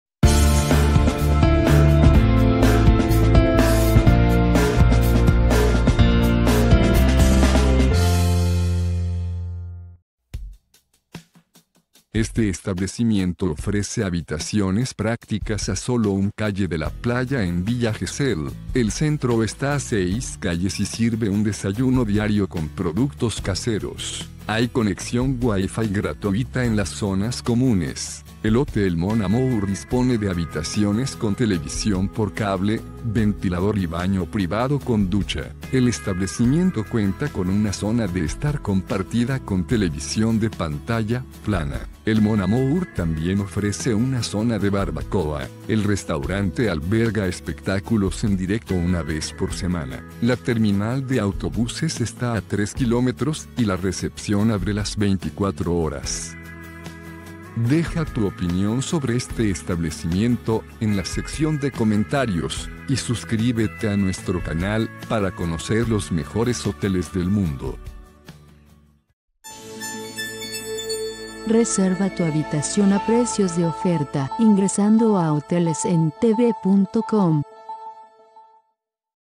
Hotel Mon Amour, Villa Gesell
Hotel Mon Amour ofrece habitaciones 2 estrellas con ducha, bañera y tv. Esta destacada edificación fue concebida para recibir turistas con diversas necesidades durante su estancia en Villa Gesell. Hotel Mon Amour tiene una puntuación de 6.7 apoyada en 42 reseñas dispensadas por los visitantes.